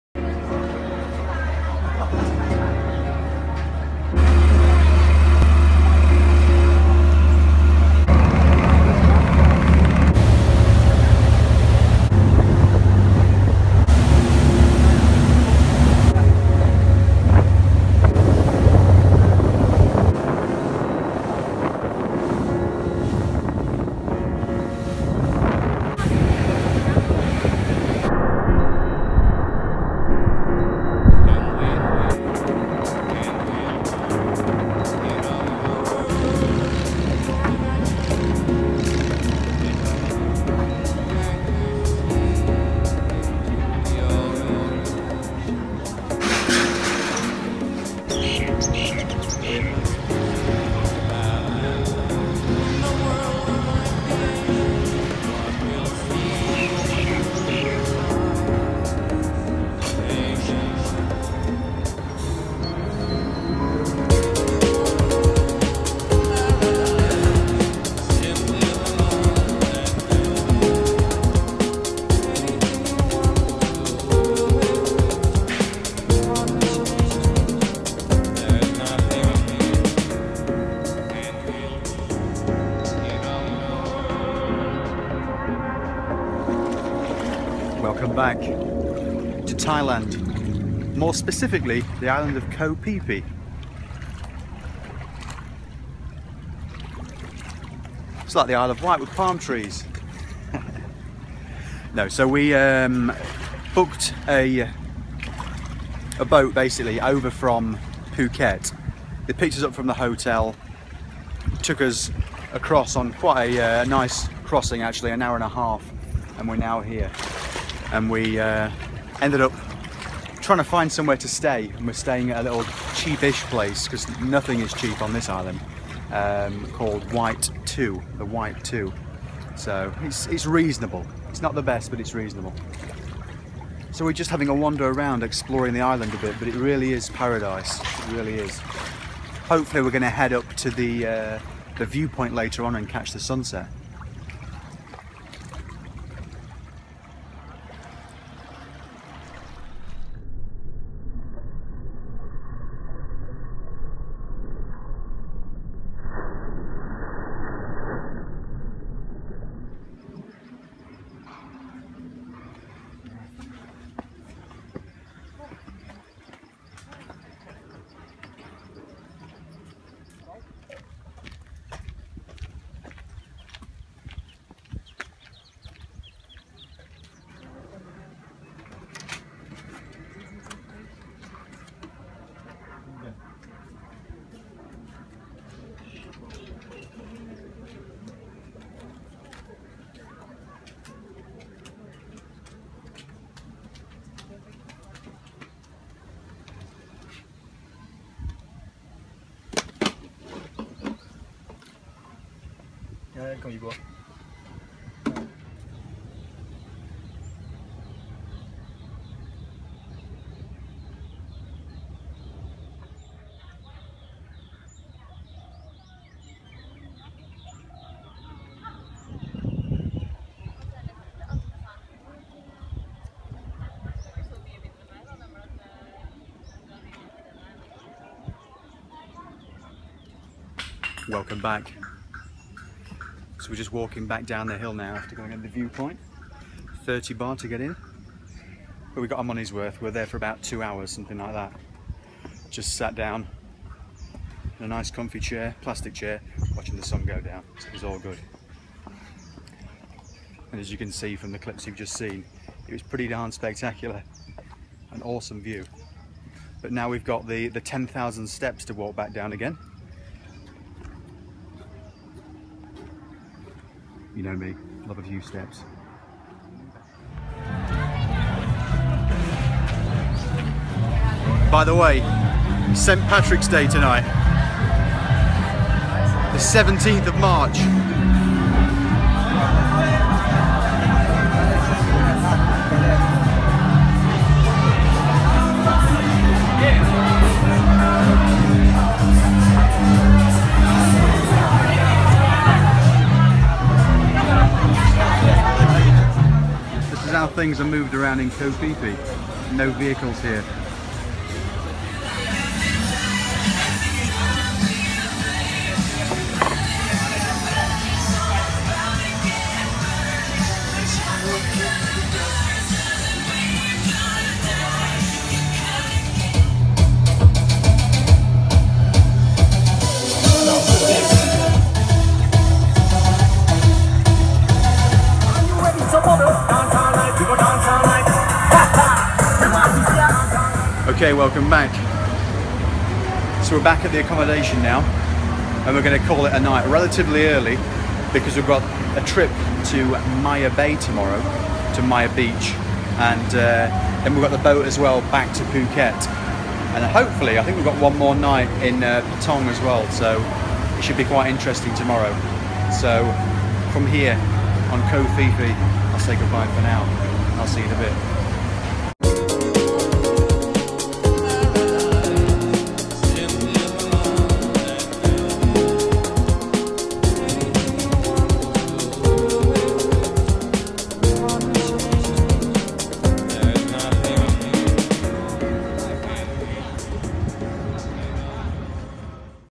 Asia Trip - Koh Phi Phi Island
Today's adventure saw us head for the beautiful island of Koh Phi Phi to spend a couple of days there. We got the ferry from Phuket and spent a great day wandering around the island, we then head up the hill to check out the spectacular views from the sunset viewpoint. Koh Phi Phi was hit by a tsunami on Boxing Day 2004 and it had been a place I'd been interested in visiting for some time, I wasn't disappointed.

Filmed and edited on an iPhone 6 Plus, Music by Dean Read.